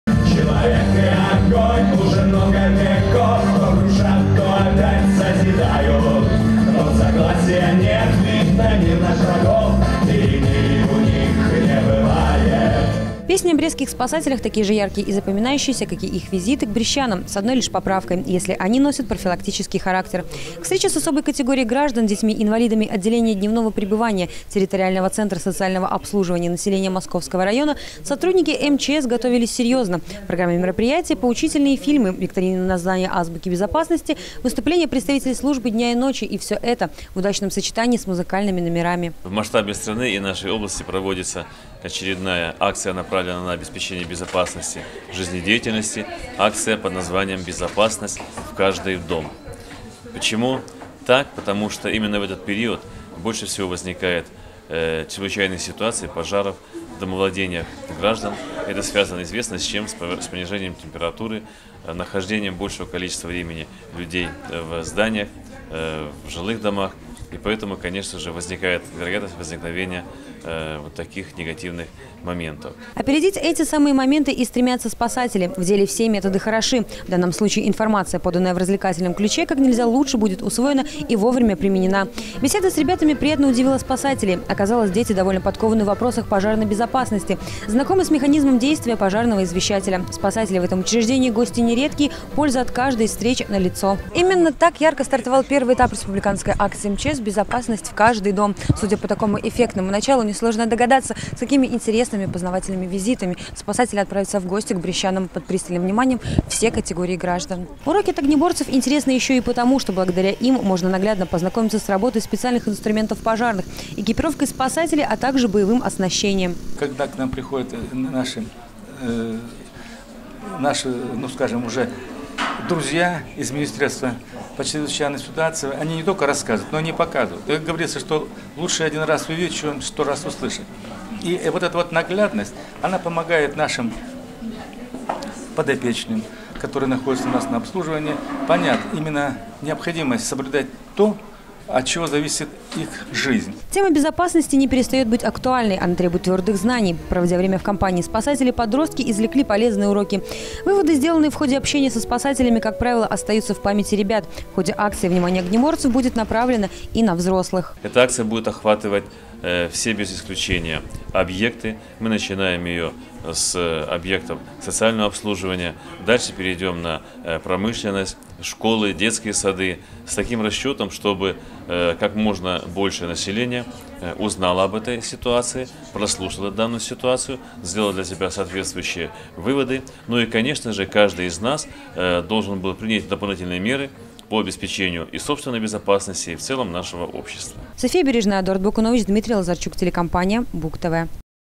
2017-02-14 г. Брест. Акция МЧС «Безопасность в каждый дом». Новости на Буг-ТВ.
В городе над Бугом стартовала республиканская профилактическая акция МЧС под названием «Безопасность в каждый дом». Как и прежде, она будет проходить в четыре этапа. В нынешнем году спасатели начали свою работу с посещения территориальных центров социального обслуживания населения. «Предупреждён – значит, вооружён!» - главный лозунг профилактического мероприятия. В целях предупреждения пожароопасных ситуаций, брестские огнеборцы отправились к воспитанникам Территориального центра социального обслуживания населения Московского района. Для особенных ребят они подготовили специальную программу.